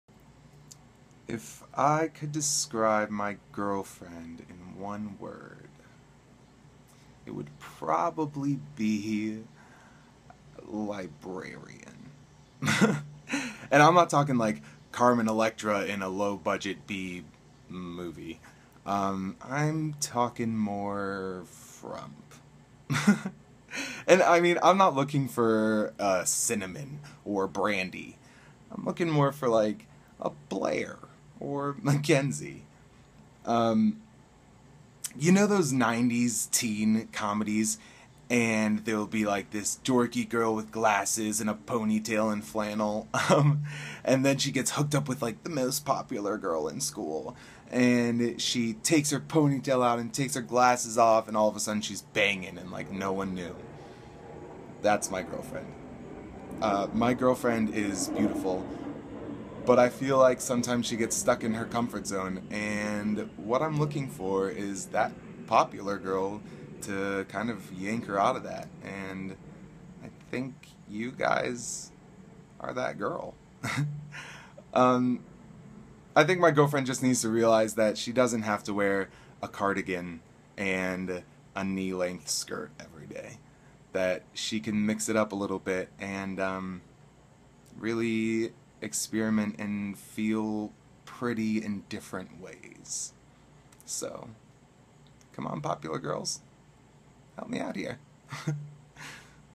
Chris Wolfe - Popular Girl (Improv)
Chris Wolfe improvising a monologue asking for a makeover for his girlfriend